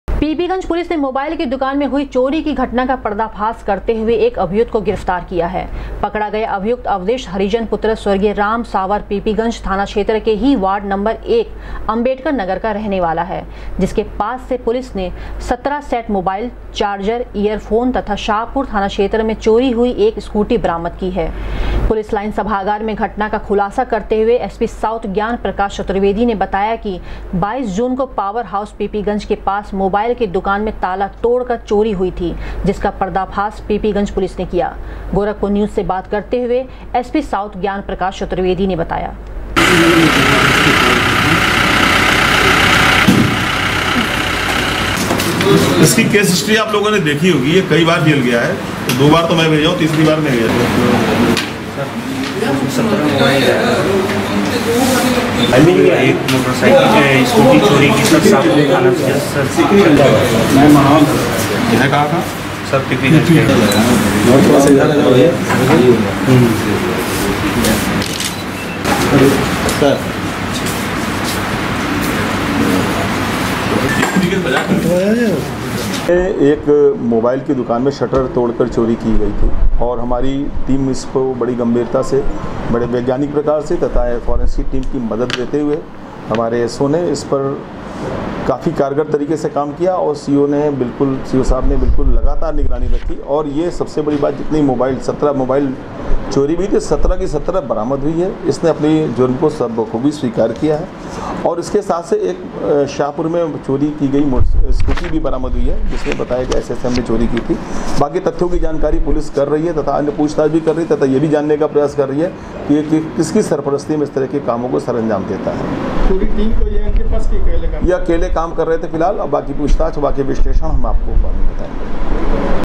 चोरी की 17 मोबाईल सेट,चार्जर ,बैटरी सहित चोरी की स्कूटी के साथ पीपीगंज पुलिस ने एक अभियुक्त
चोरी की 17 मोबाईल सेट,चार्जर ,बैटरी सहित चोरी की स्कूटी के साथ पीपीगंज पुलिस ने एक अभियुक्त को दबोचा, एसपी साऊथ ने किया खुलासा